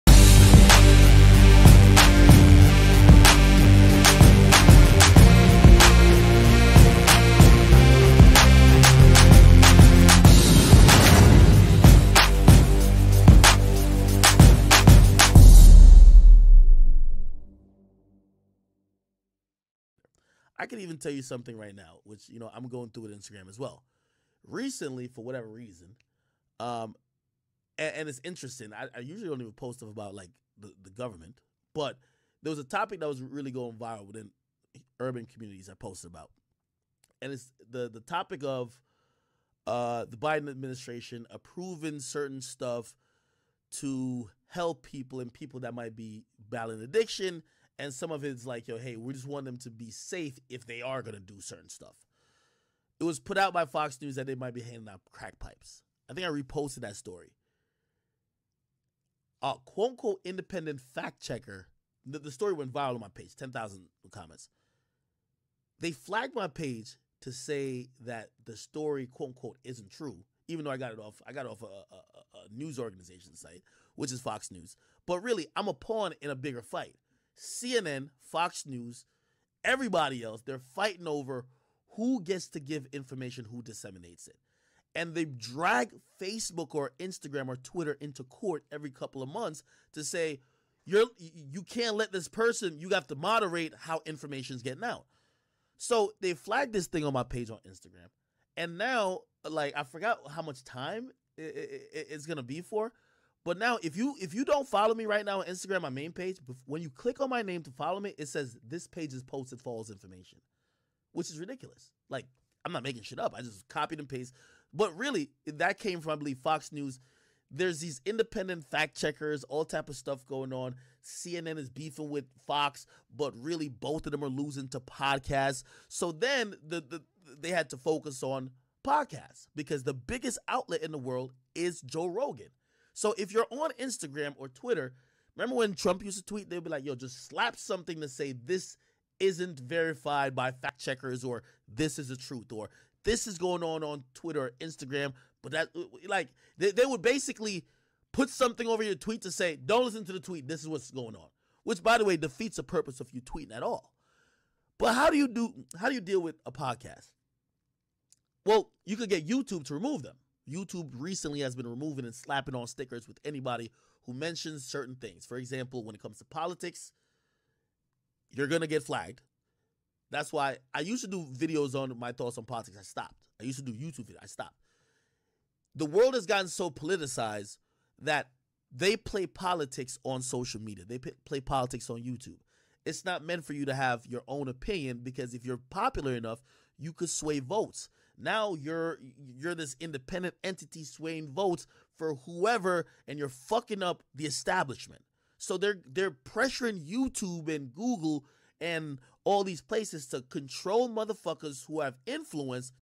DJ Akademiks Speaks On Censorship and How The Democrats And Corporations Tried To Silence Him
IF YOU WANT YOUR BEATS AND SONGS ON SPOTIFY AND ALL MUSIC STREAMING PLATFORMS